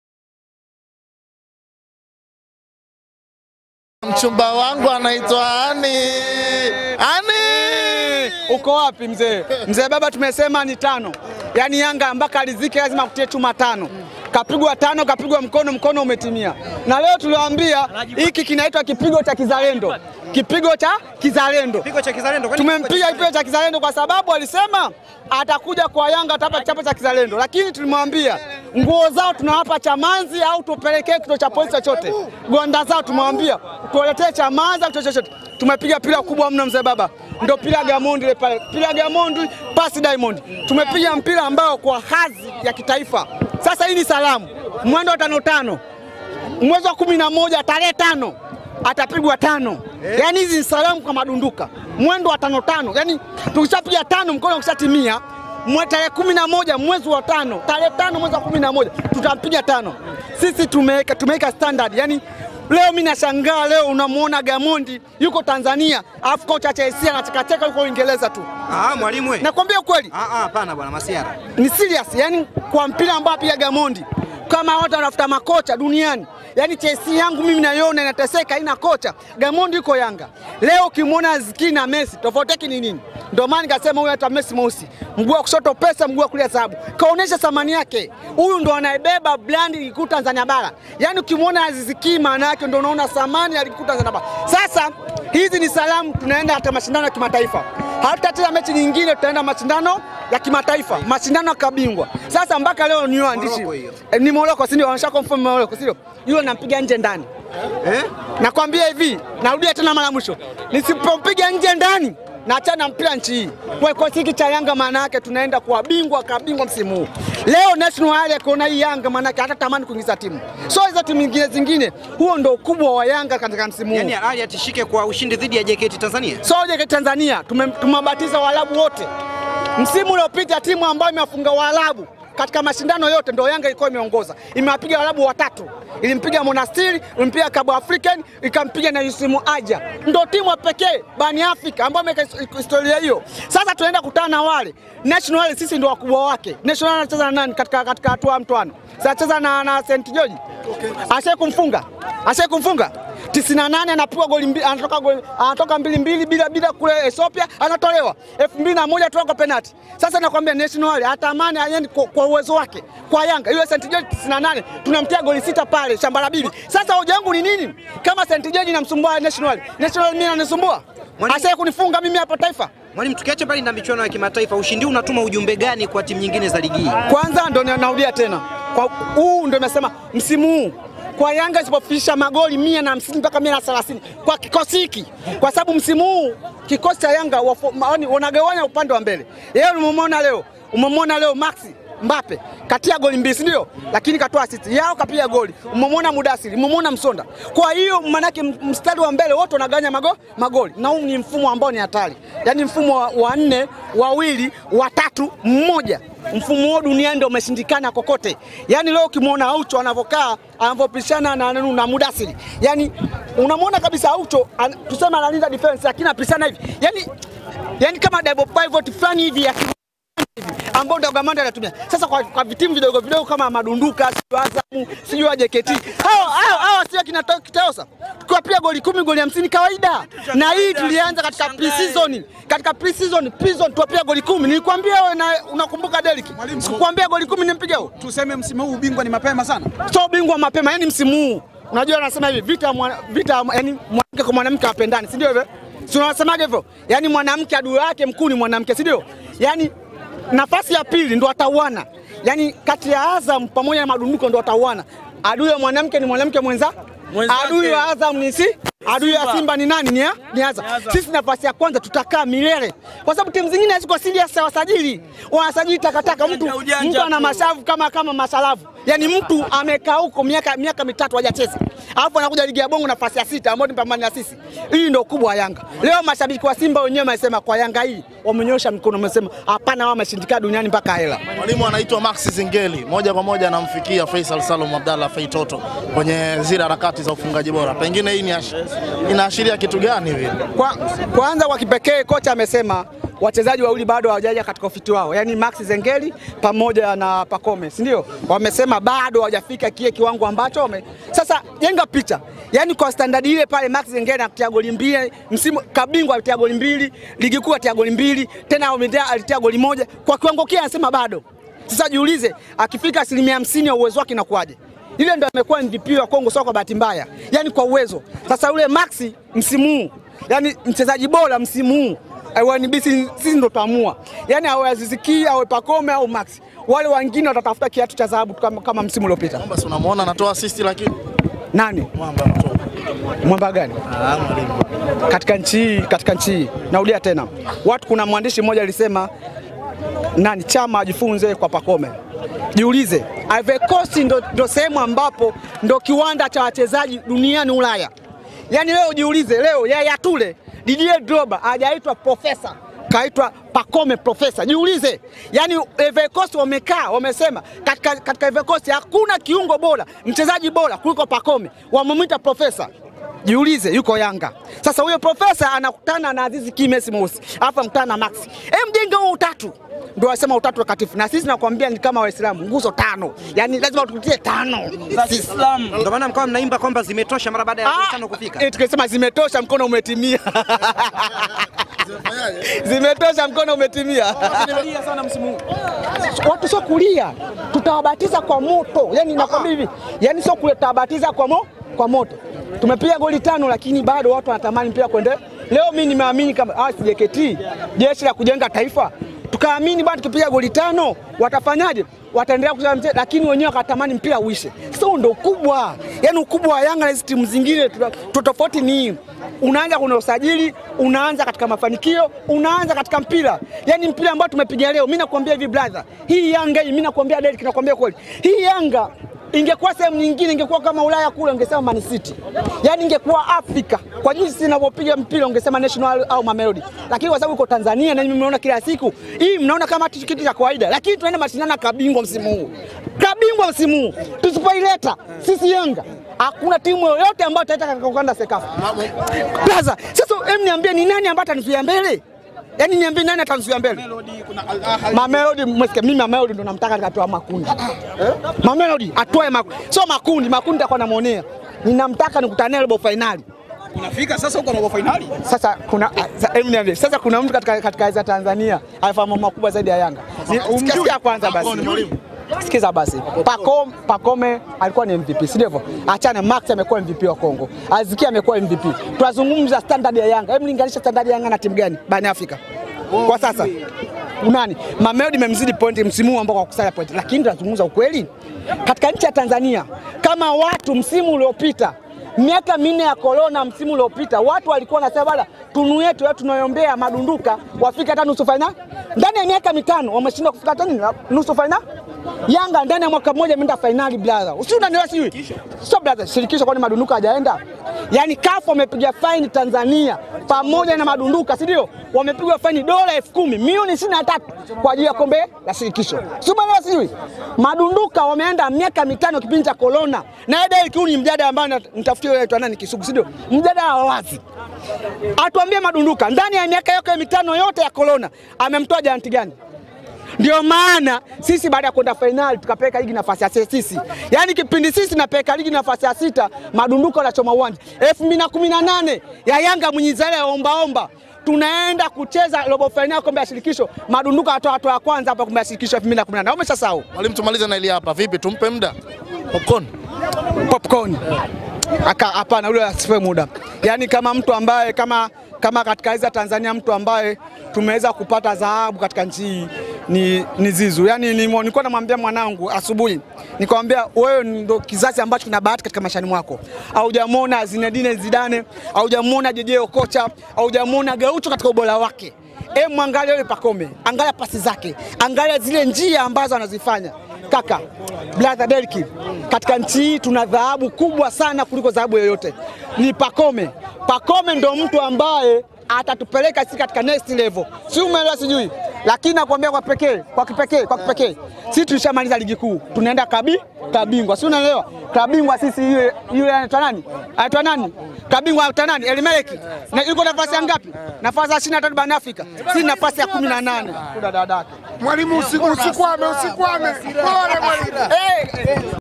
🔴MWALIMU YANGA AVURUGWA NA USHINDI WA 5G, AJE KOLO tupe 5G ! HII NI YANGA, TUMEWAPIGA MKONO JKT
🔴MWALIMU YANGA AVURUGWA NA USHINDI WA 5G! AJE KOLO AKULA 5G ! HII NI YANGA, TUMEWAPIGA MKONO JKT